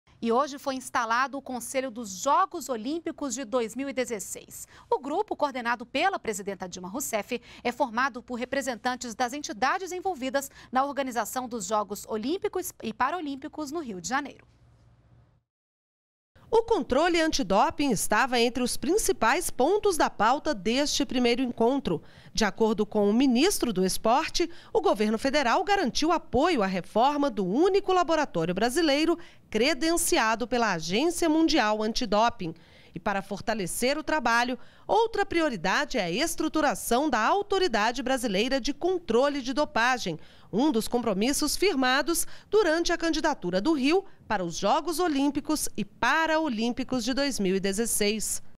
Conselho dos Jogos Olímpicos é instituido e quer reforçar o combate ao doping
NBR NOTÍCIAS - 19.09.12: O Conselho dos Jogos Olímpicos de 2016 foi oficialmente instalado, nesta quarta-feira (19). O grupo terá coordenação da presidenta Dilma Rousseff e será composto por representantes de entidades envolvidas na organização dos Jogos Olímpiocos e Paralimpicos, no Rio de Janeiro. Na discussão do Conselho, o governo federal garantiu a reforma do único laboratório brasileiro credenciado pela Agência Mundial Antidoping. Outra prioridade afirmada será a estruturação da autoridade brasileira de controle de dopagem.